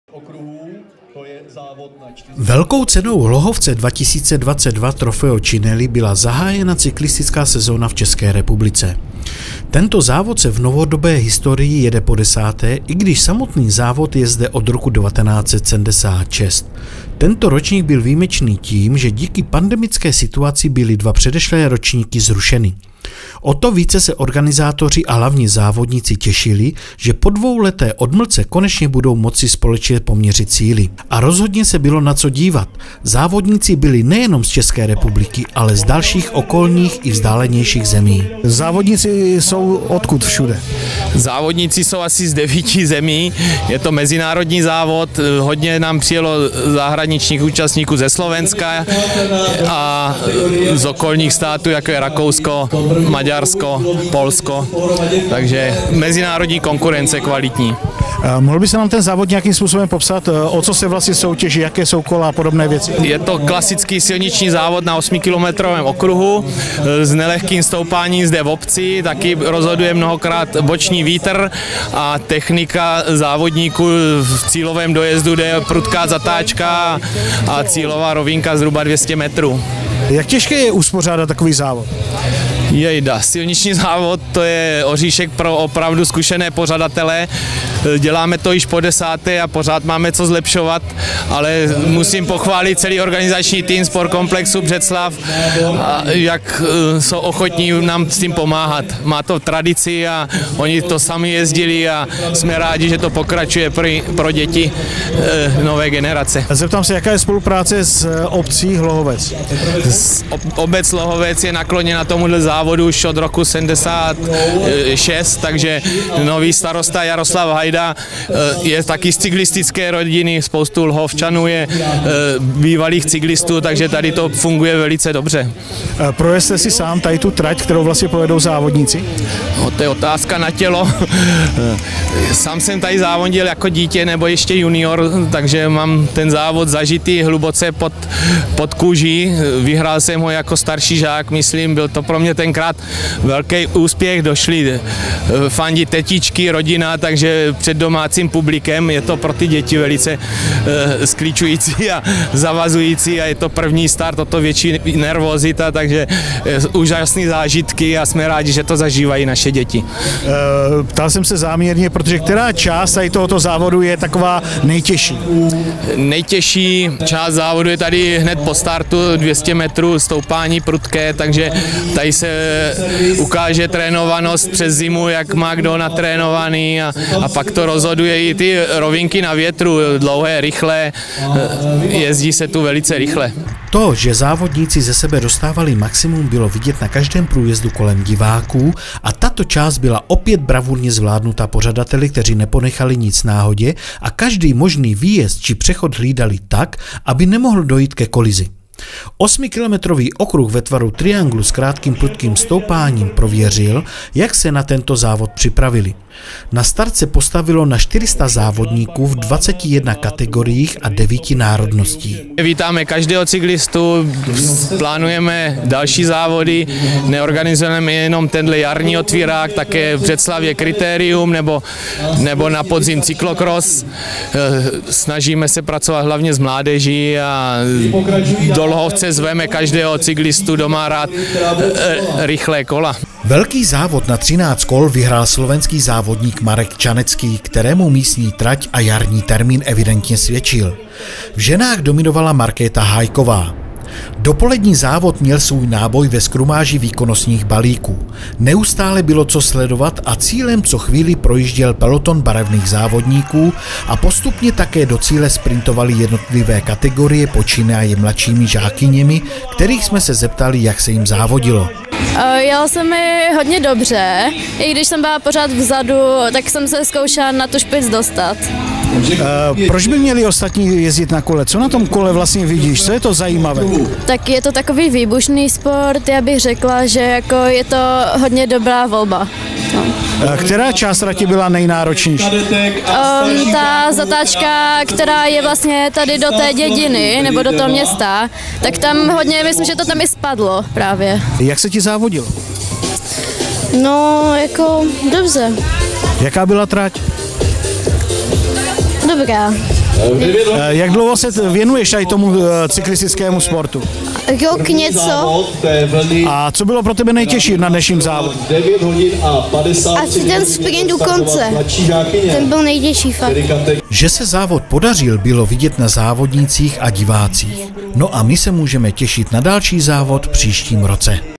Cyklistické závody v České republice zahájeny velkou cenou Hlohovce Trofeo Cinelli
Velkou cenou Hlohovce 2022 Trofeo Cinelli byla zahájená cyklistická sezona v české republice, tento závod se v novodobé historii jede podesáté i když samotný závod je zde od roku 1976, tento ročník byl výjimečný tím, že díky pandemické situaci byla dva předešlé ročníky zrušeny. O to více se organizátoři, a hlavně závodníci těšili, že po dvouleté odmlce konečně budou moci společně poměřit síly. A rozhodně se bylo na co dívat, závodníci byly nejenom z české republiky, ale z dalších okolních i vzdálenějších zemí.